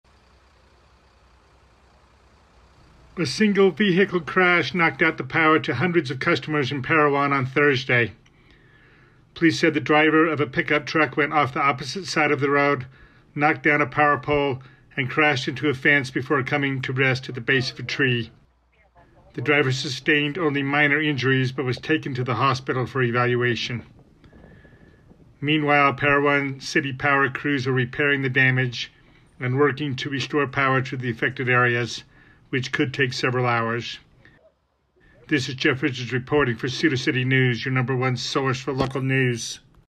Single-vehicle crash knocks out power to hundreds of customers in Parowan
PAROWAN — A single-vehicle crash knocked out power to hundreds of customers in Parowan late Thursday morning, authorities said.
The incident, which was reported at 11:41 a.m., involved a white Dodge Ram pickup truck belonging to a local electrical contractor.
Written by Jeff Richards
August 3, 2023